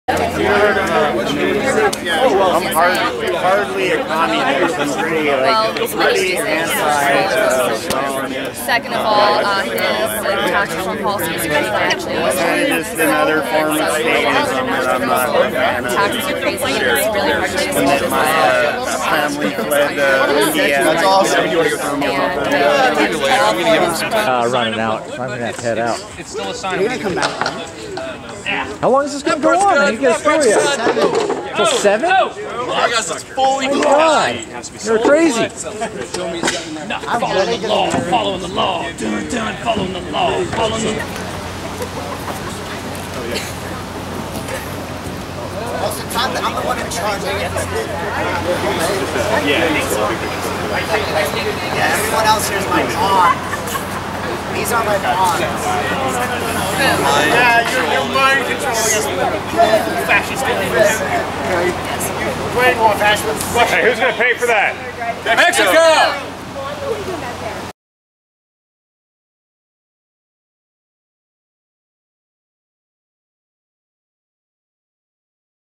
By Popular Demand: The Blonde Pro Trump Chick
Lots of people were commenting about her on the "triggliots" video, so here she is starring in her own video.